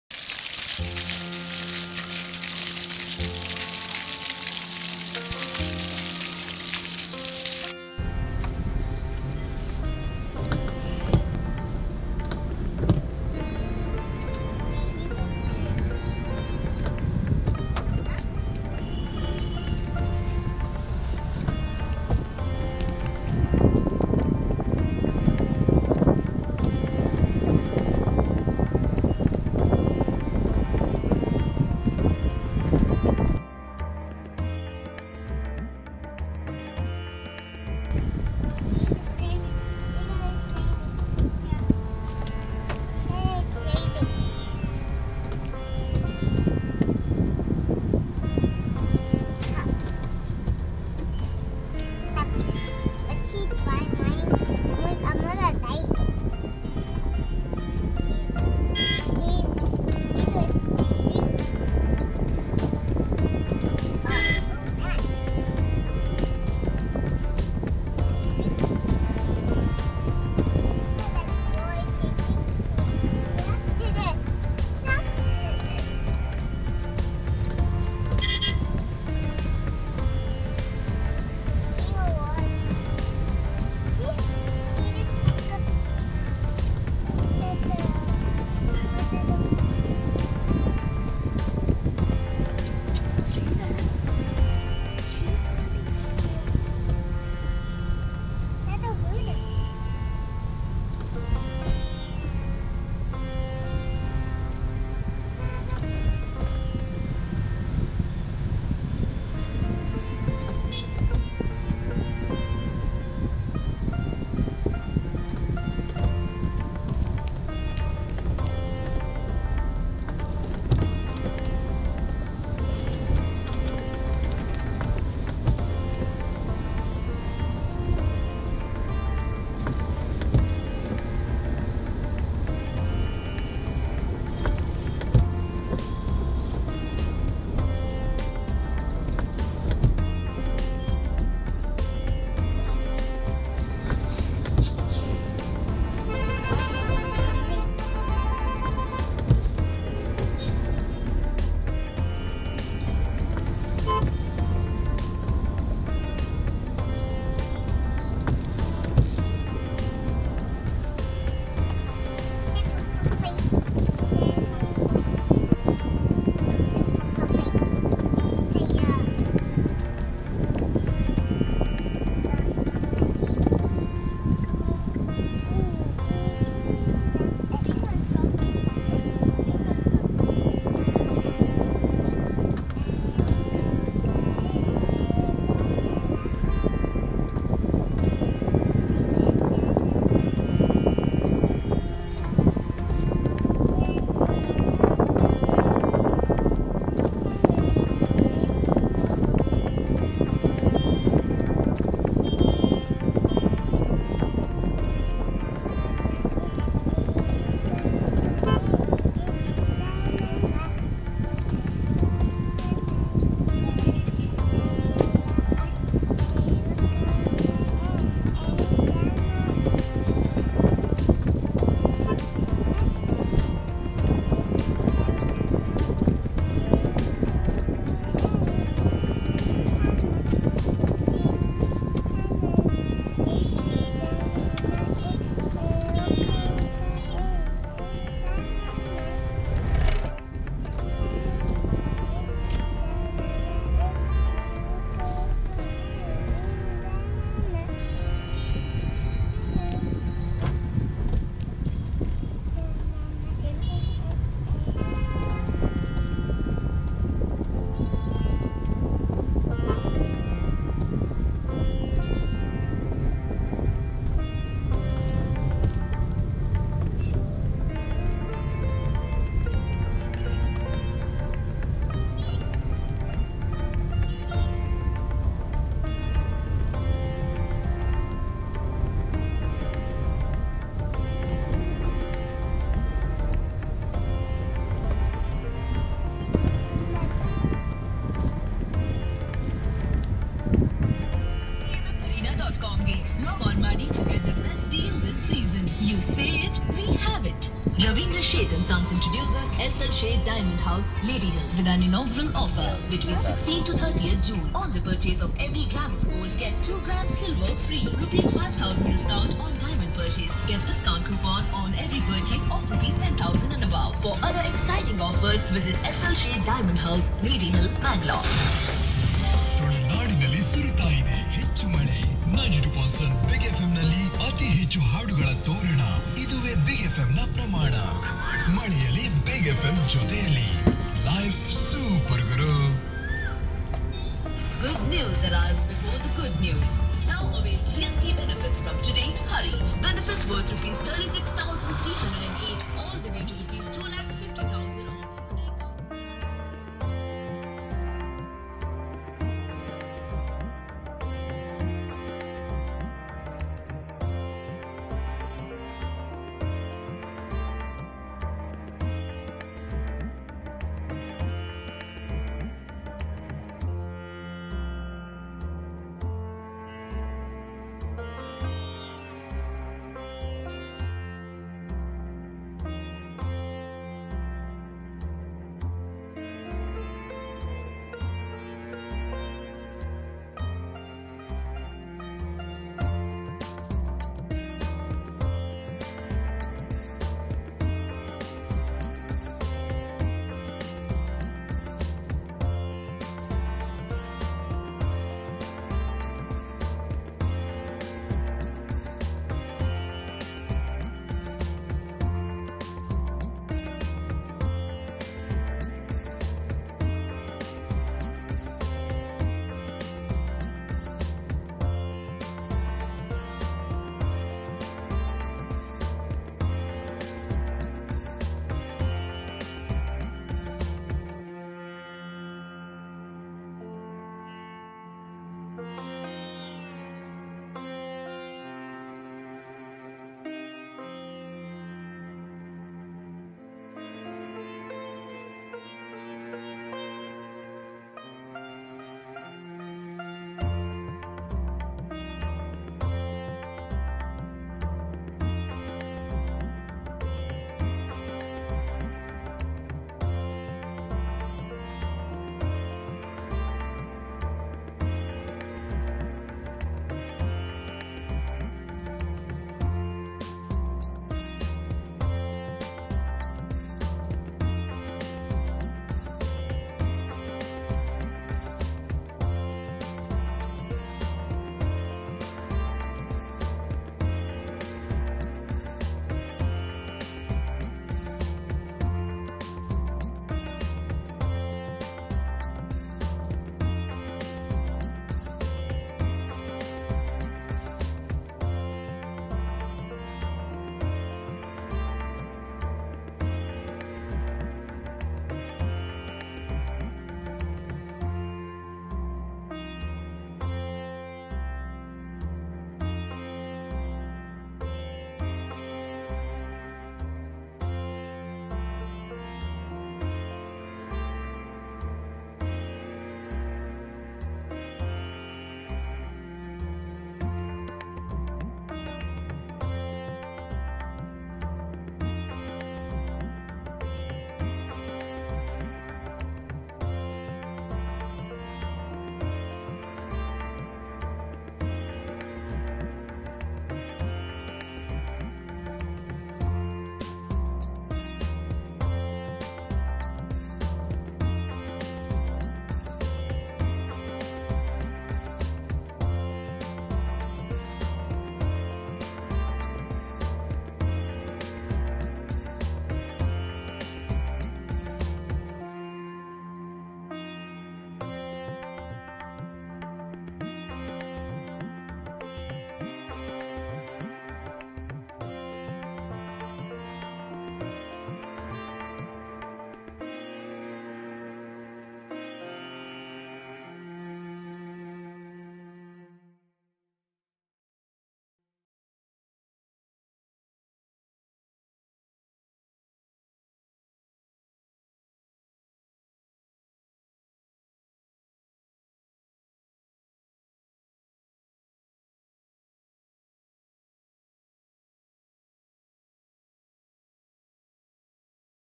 Rainy days in Mangalore for cuddling & watching it from car is fun !!
❤️ Stories from 2 STATES - SF2
Hi, This is our vlog 2 STATES and will update you guys with family, Films, Art, Cuisine, and lifestyle videos.